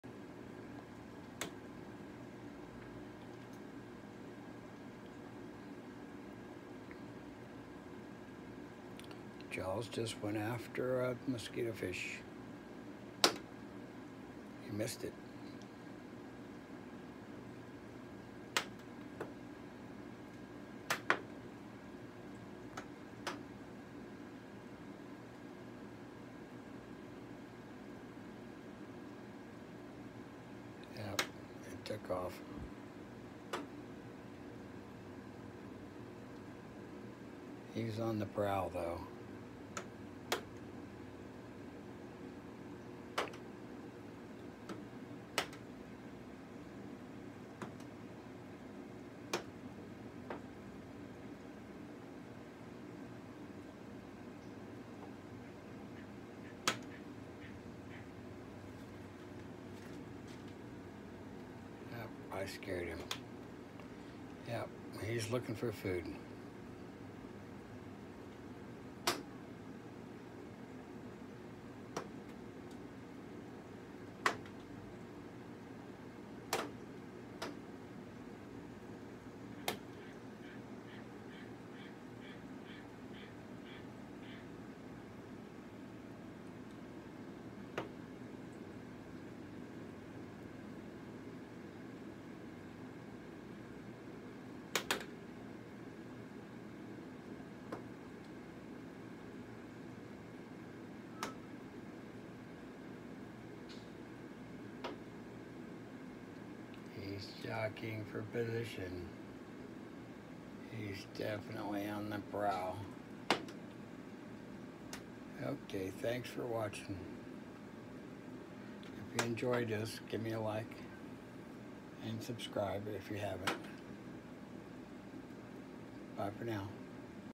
JAWS # I …IS ON THE PROWL FOR A MEAL. HE WENT AFTER A GAMBUSIA .
Jaws￼ # I ..Is on the prowl for a meal, but he missed it. The time is near for me to get that on video . I can feel it.